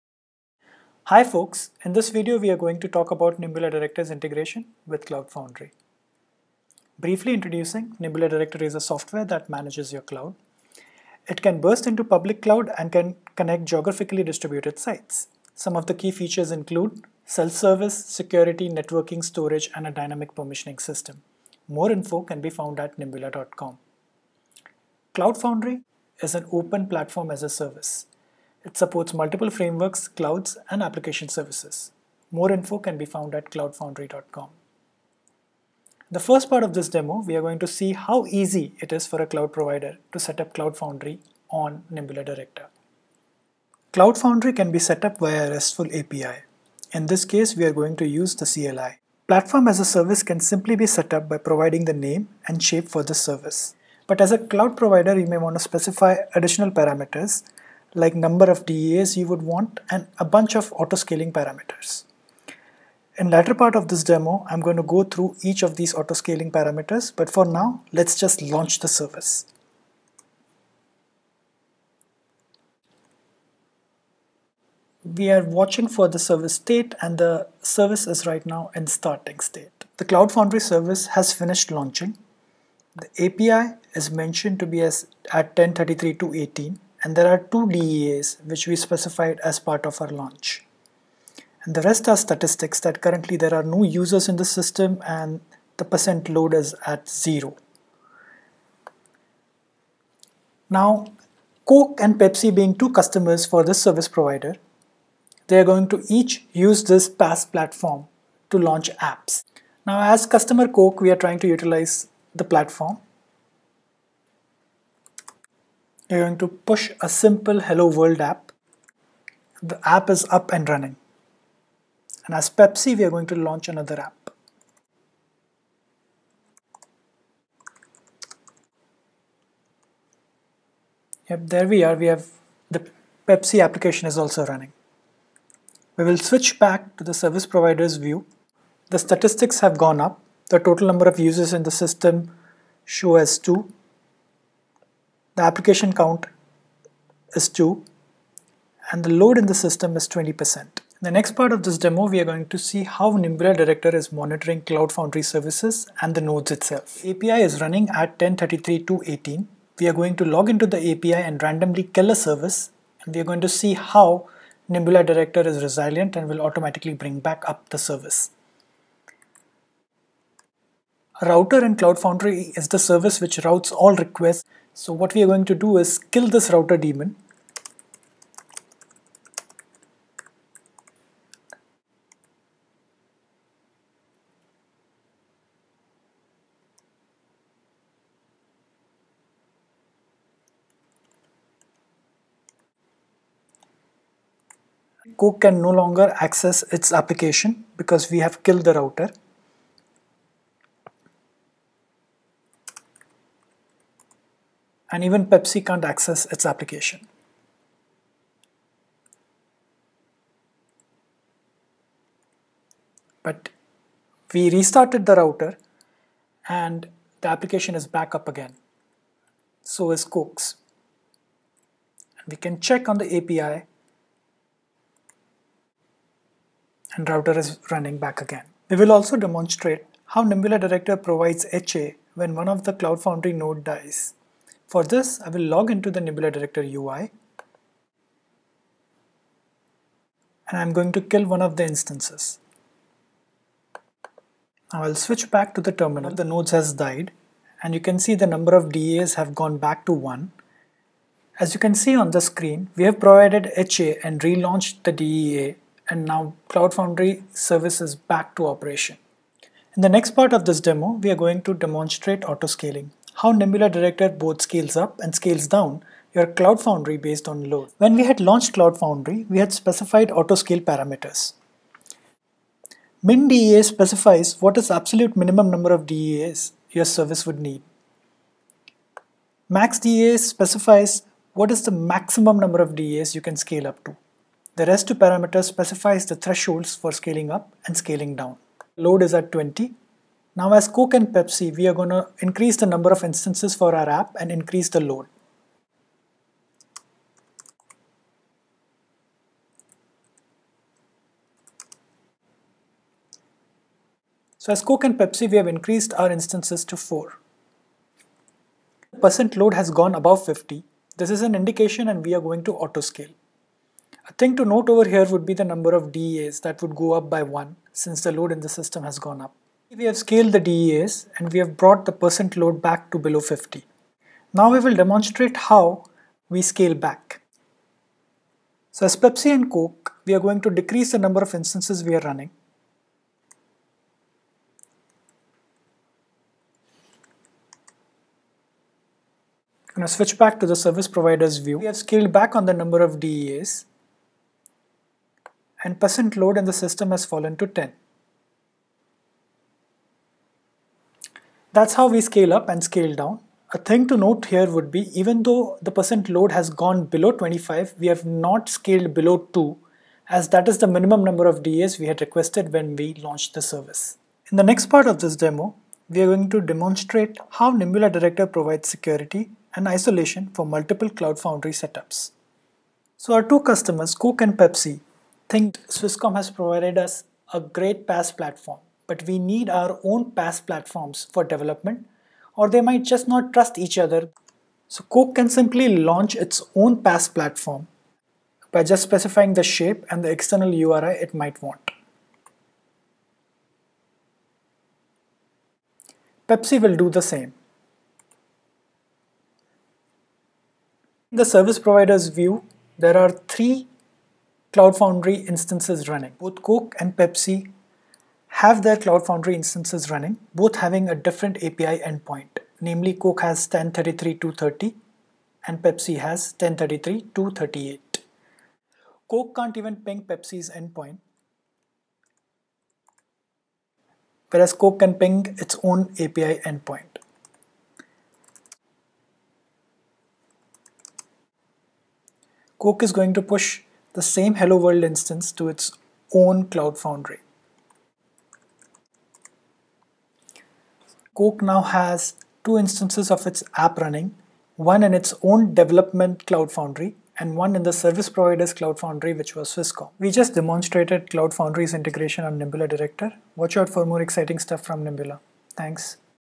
Cloud Foundry with Nimbula Director
This video talks demonstrates Nimbula Director's integration with Cloud Foundry.

1. Push Button Setup (Instant setup via API and CLI)
2. Resilience and High Availability(HA)
3. Auto Scaling (both scale up and scale down)
4. Security (Multiple PaaS setups on the same cloud providing isolation and security)